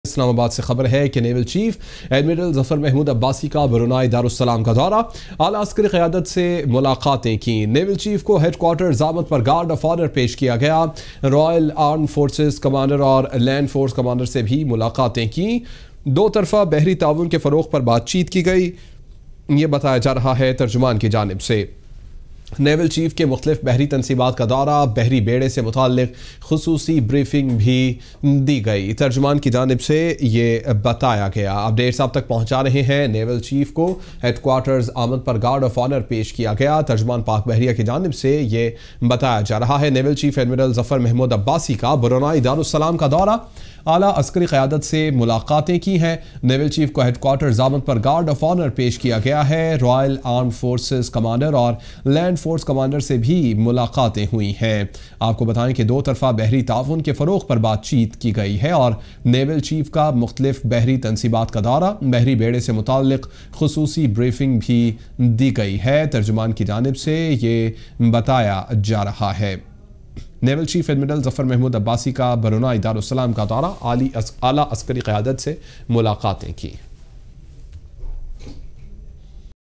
Naval Chief Admiral Zafar Mahmood Abbasi pays an official visit to Brunei Darussalam
نیول چیف ایڈمرل ظفر محمود عباسی نے برونائی دارالسلام کا سرکاری دورہ کیا